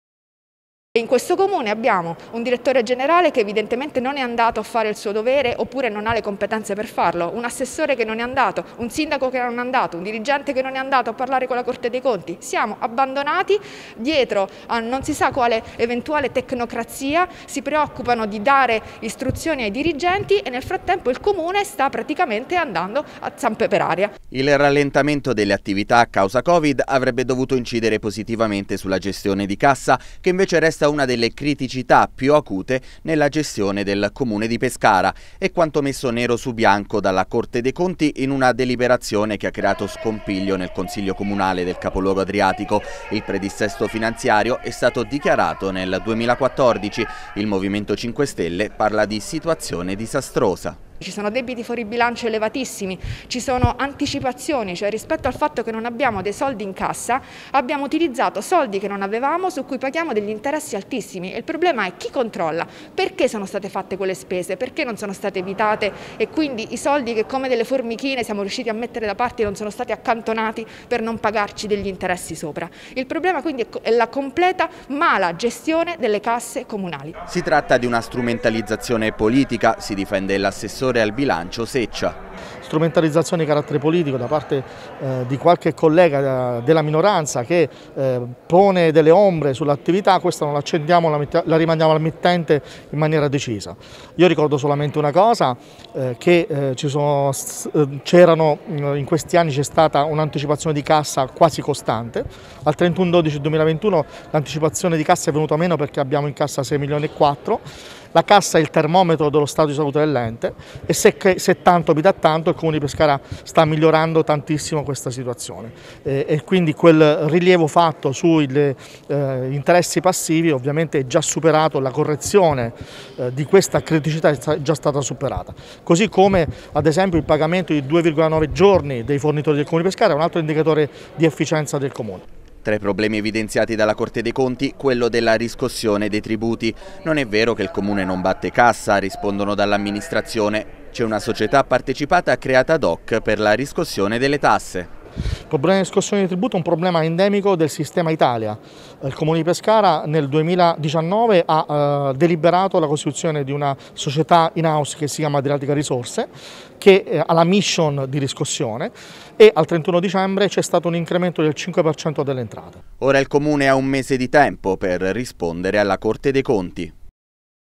Pescara. La corte dei conti bacchetta il Comune
Una deliberazione della corte dei conti punta il dito contro la gestione di cassa del Comune di Pescara. Caos in Consiglio comunale, proprio mentre si discute il bilancio.